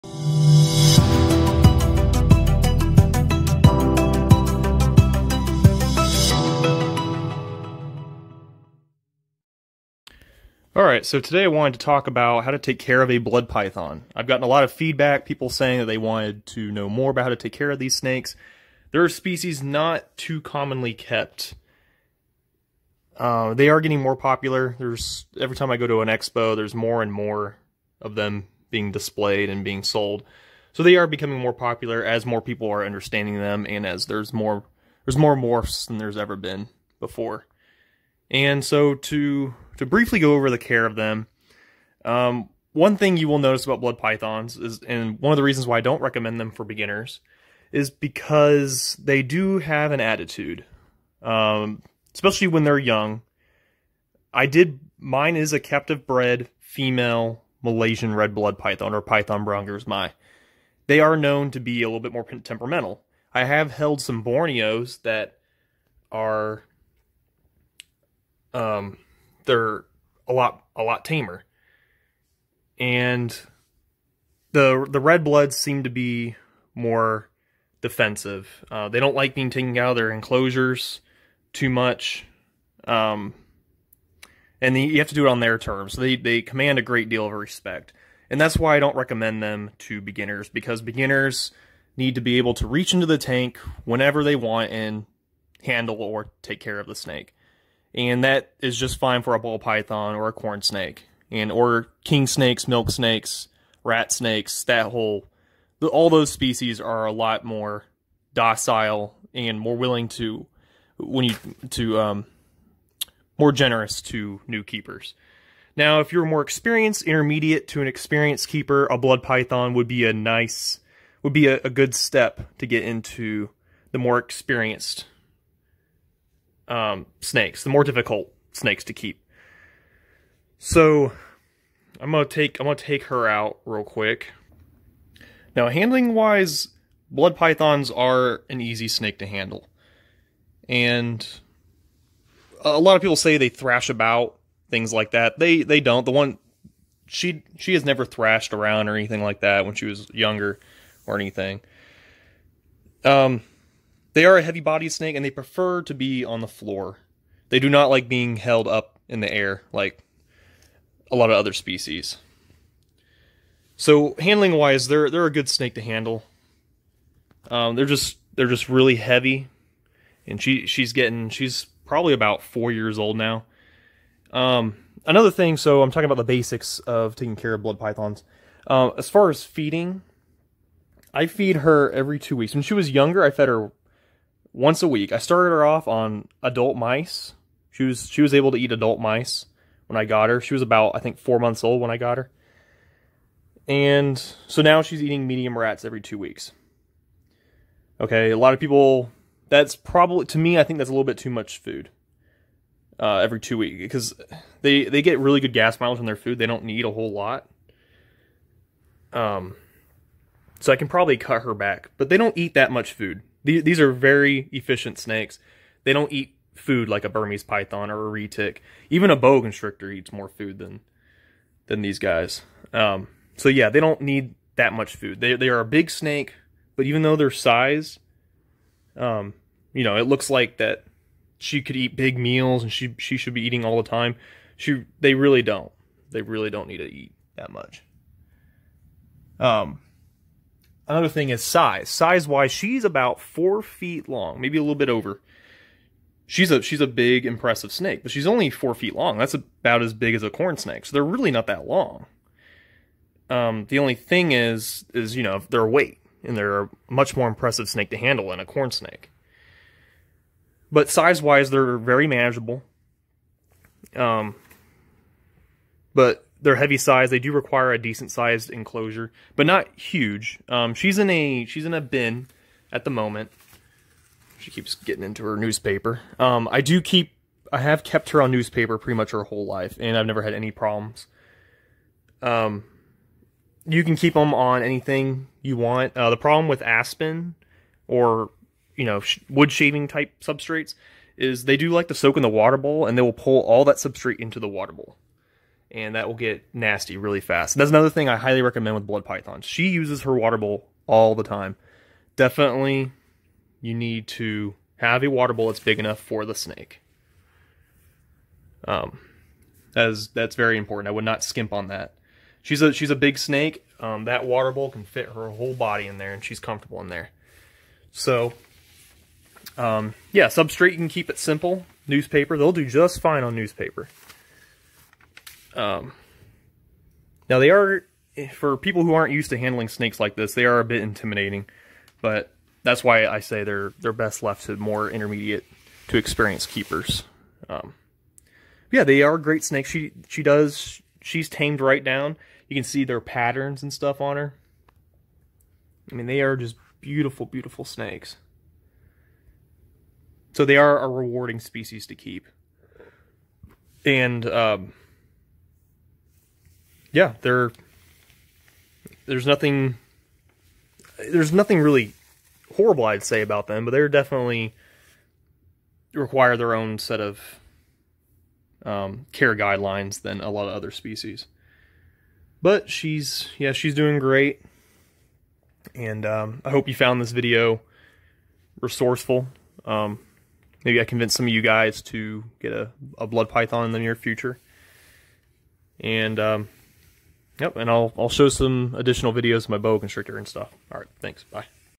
How to Care for a Blood Python
Care requirements for a blood python. Temperament, feeding, size, and substrate.